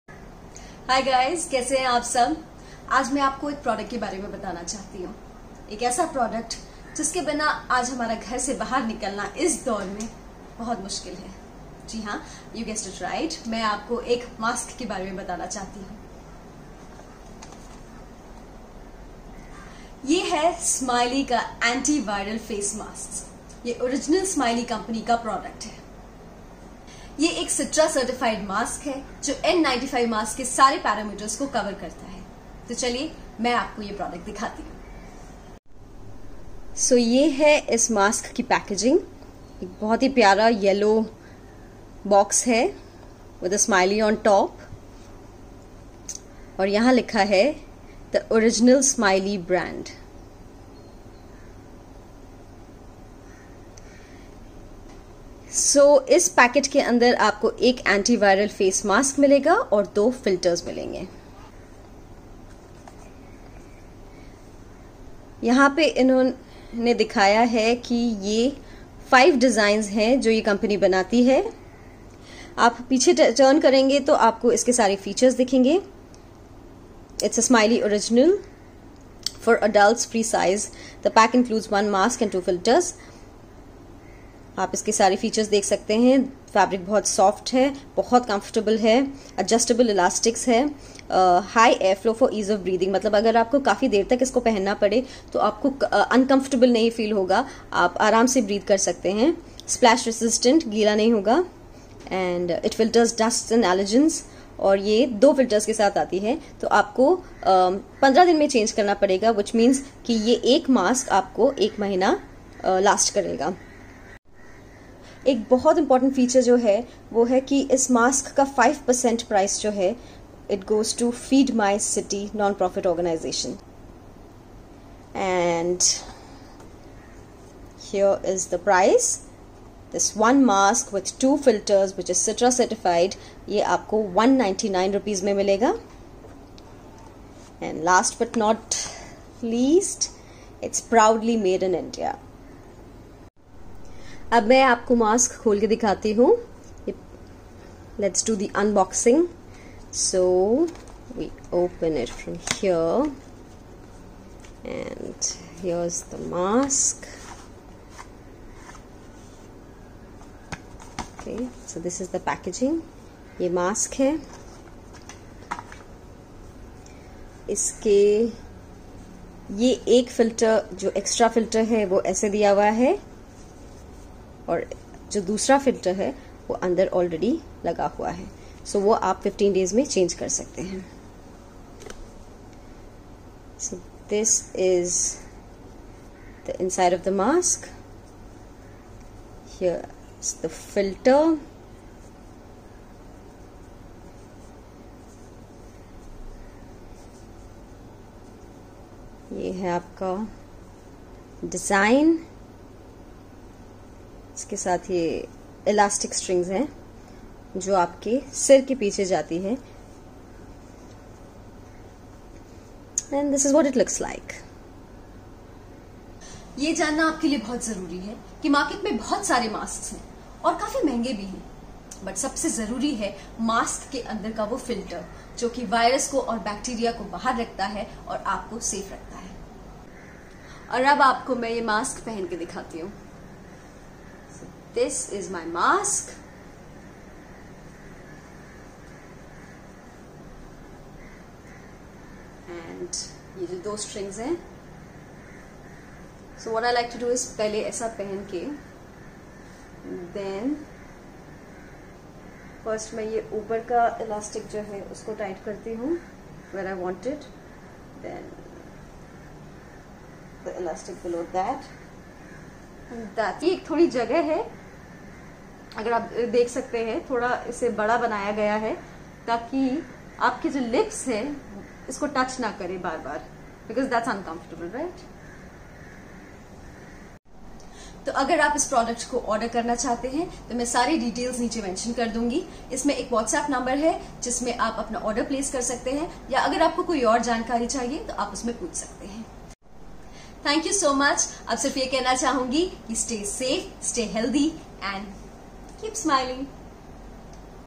The best face mask for protection against Corona - UNBOXING & REVIEW IN HINDI
This is a smiley Anti viral mask , a product of the original smiley brand . It has two filters in a pack and one mask and the filters are certified by SITRA . It gives the same protection as an N95 mask . It comes  in 5 different designs. Each mask is for 199 and 5 per cent of the price goes to a non profit organization that feeds the poor .

Get these masks for yourself , for your family & friends or Donate it to your staff or people who work for you so that you can protect yourself and them !!